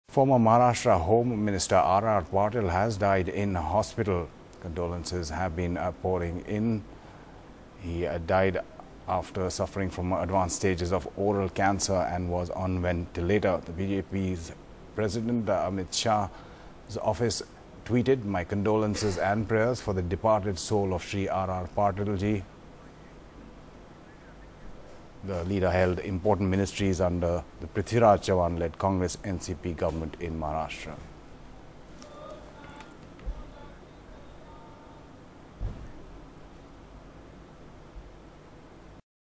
RR Patil, former Maharashtra Home Minister, dies at 57
RR Patil, the former Deputy Chief Minister of Maharashtra, has died at 57. He was being treated at Mumbai's Lilavati hospital for cancer. Mr Patil, six-term MLA from Tasgaon in Sangli district, is survived by his mother, wife, and two daughters.